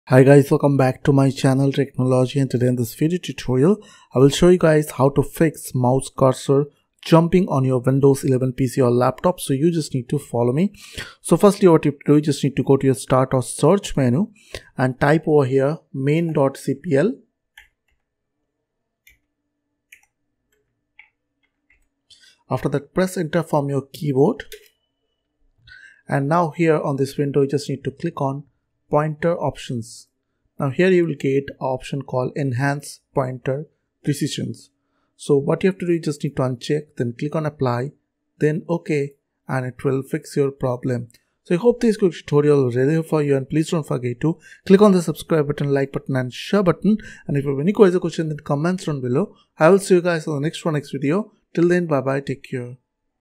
How to Fix Mouse Cursor Jumping in Windows 11
Here in this video tutorial, I will show you guys how to fix mouse cursor jumping automatically in Windows 11.